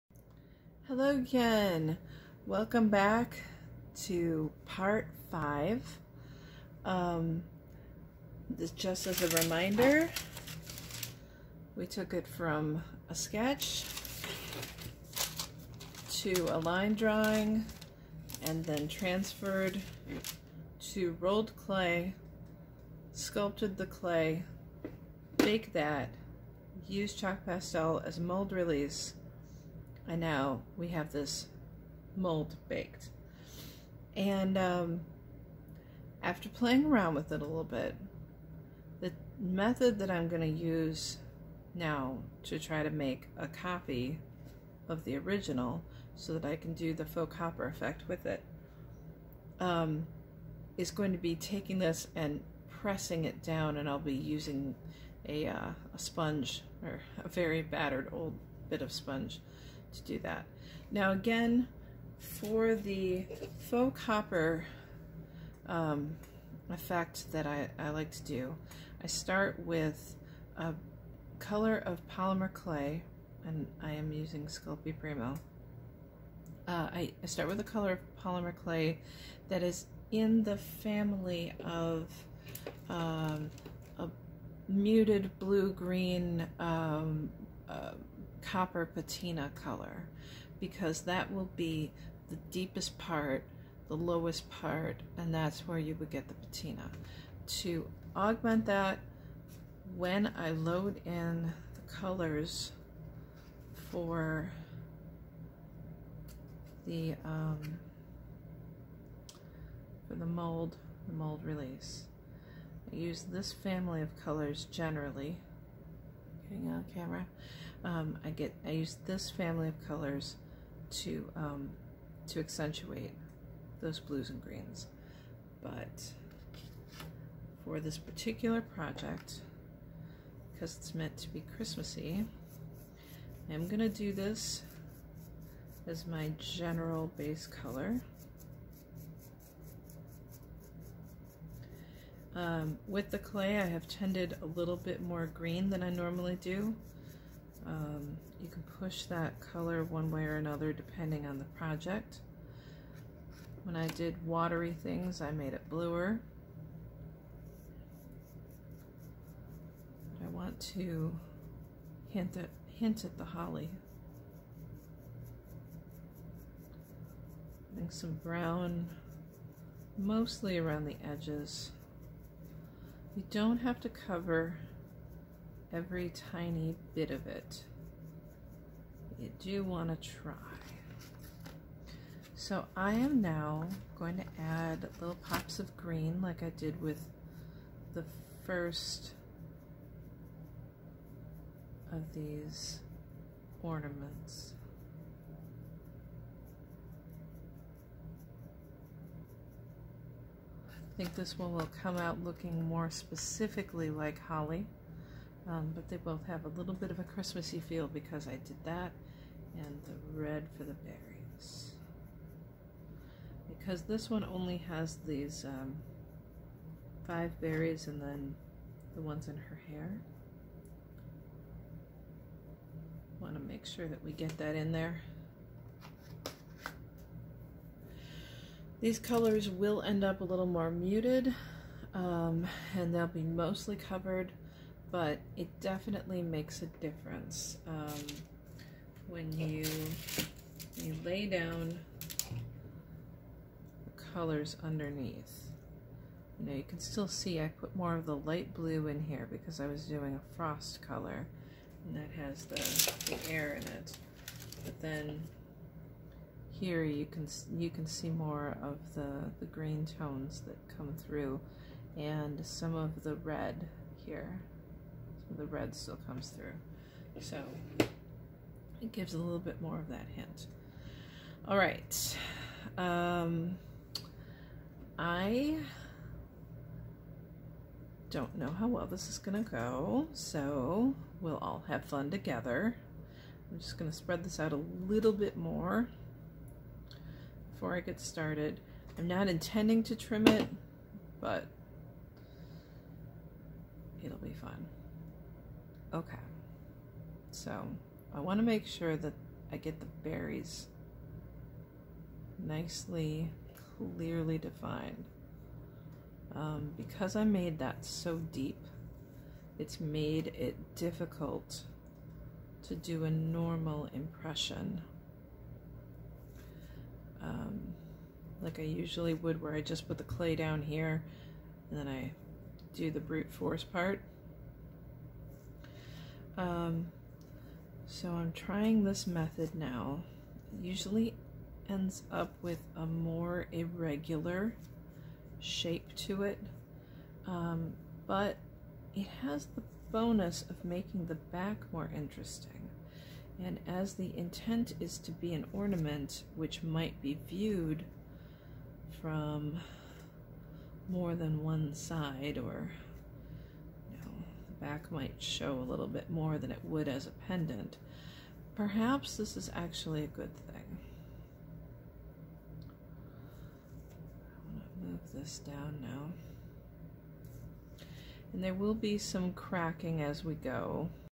Holly Ornament #2, part 5
Part 5 of 6 in a series of videos showing how I make a polymer clay ornament from sketch to completion.  This video cuts off awkwardly.  If you're following along with your own project, just keep pressing clay into the mold, but don't let it get too thin.